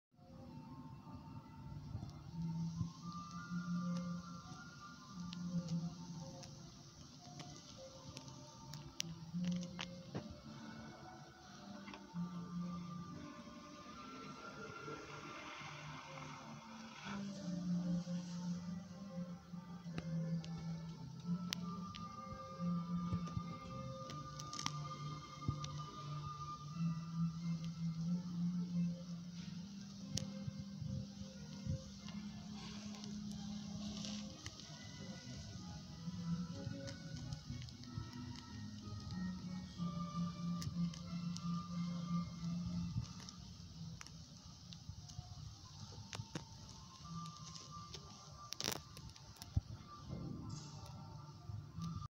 Jaakko Mäkikylä
3/3  Artistic weapons at the MOCA, Museum of Contemporary Art, Bangkok.

Bangkokin modernin taiteen museossa näin hienoja teräaseita. Niistä ei oiken saanut kuvaa vitriinissä, mutta video onnistui.
Näitä on kolme videota.